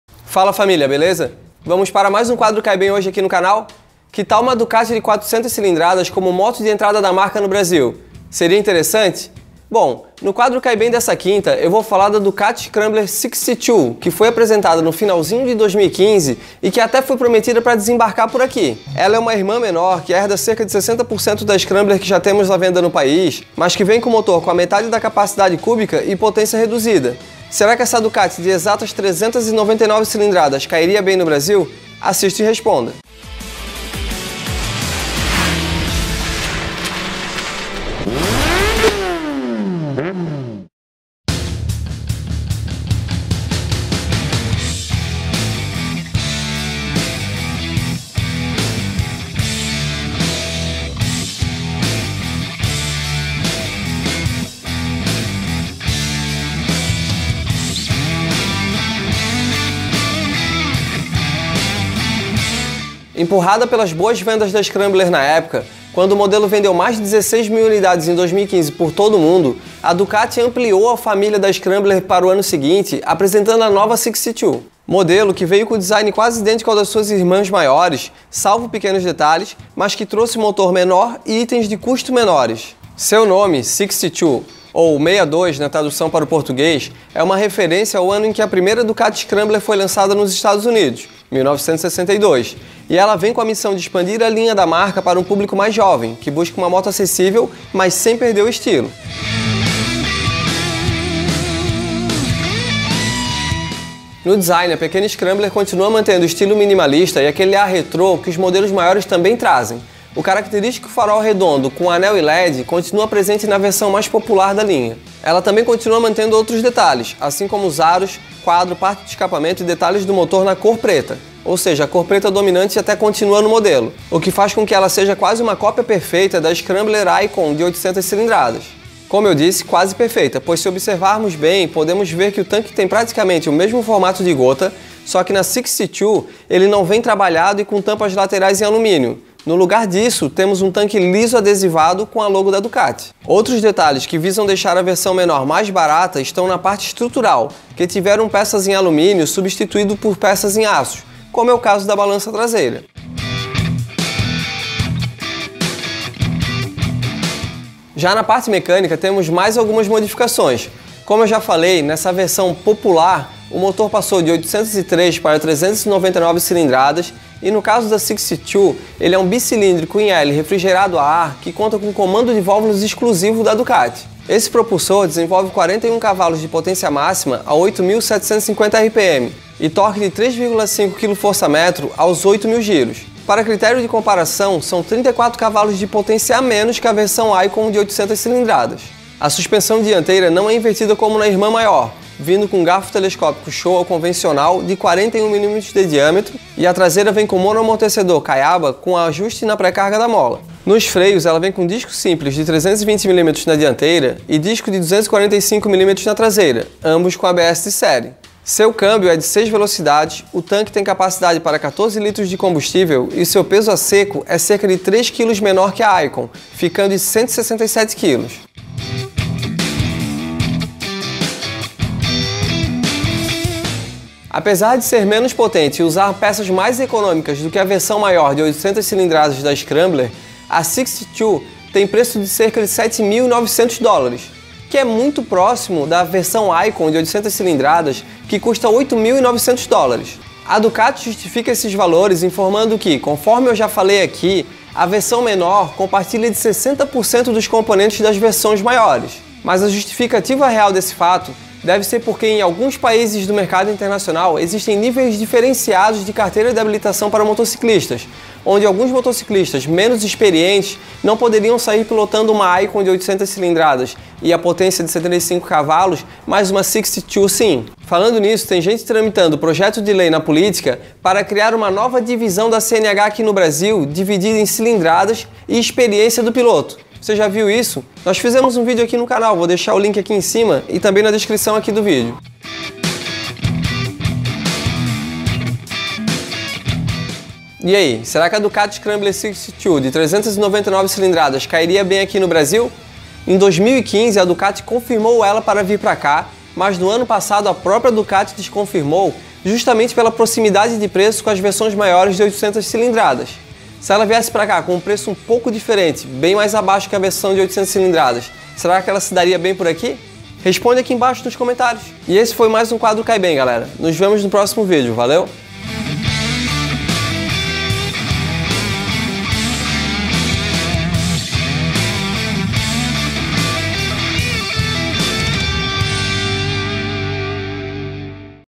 CAI BEM? DUCATI DE 399CC CAIRIA BEM NO BRASIL? - Motorede #14
A Ducati Sixty2 de 399cc cairia bem no Brasil? Assista e responda!

A Ducati Scrambler Sixty2 (62 em inglês) foi apresentada ao mundo no final de 2015 e até foi prometida para desembarcar por aqui.

Ela é uma irmã menor que herda cerca de 60% da Scrambler que já temos a venda no país, mas que vem com motor com a metade da capacidade cúbica e potência reduzida. Será que essa Ducati de exatas 399cc cairia bem no Brasil? Assista e responda!

Empurrada pelas boas vendas da Scrambler na época, quando o modelo vendeu mais de 16000 unidades pelo mundo em 2015, a Ducati ampliou a família da Scrambler para o ano seguinte apresentando a nova Sixty2.

Modelo que veio com design quase idêntico ao das suas irmãs maiores, salvo pequenos detalhes, mas que trouxe motor menor e itens de custo menores.

Seu nome, Sixty2 ou “62” na tradução para o português, é uma referência ao ano em que a primeira Ducati Scrambler foi lançada nos Estados Unidos, 1962, e ela vem com a missão de expandir a linha da marca para um publico mais jovem, que busca uma moto acessível, mas sem perder o estilo.

Será que ela cairia bem no Brasil? Assista e responda!